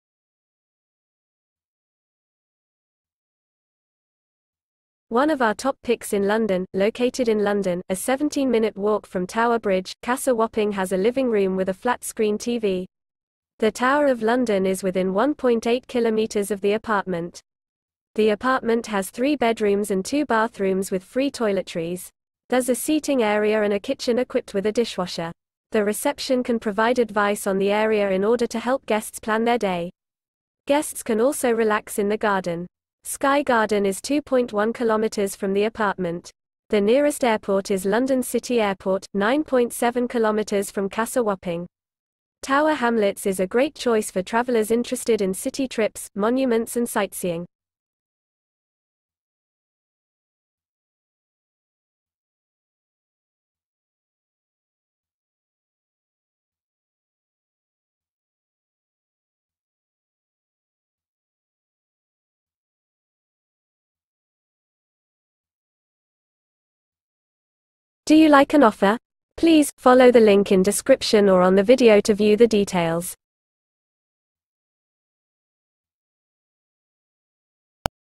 Casa Wapping | Review Hotel in London, Great Britain
One of our top picks in London.Located in London, a 17-minute walk from Tower Bridge, Casa Wapping has a living room with a flat-screen TV. The Tower of London is within 1.8 km of the apartment. The apartment has 3 bedrooms and 2 bathrooms with free toiletries. There's a seating area and a kitchen equipped with a dishwasher. The reception can provide advice on the area in order to help guests plan their day. Guests can also relax in the garden. Sky Garden is 2.1 km from the apartment. The nearest airport is London City Airport, 9.7 km from Casa Wapping. Tower Hamlets is a great choice for travelers interested in city trips, monuments and sightseeing.